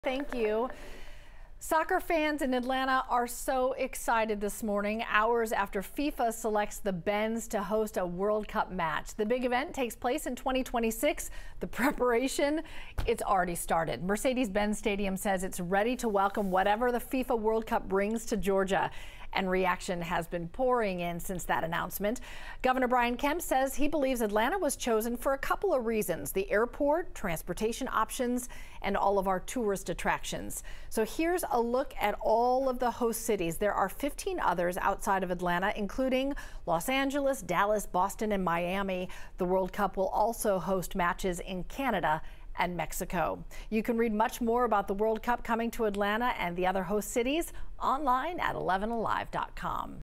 Atlanta chosen as a World Cup host city
It's one of 11 U.S. cities that will see World Cup games in 2026.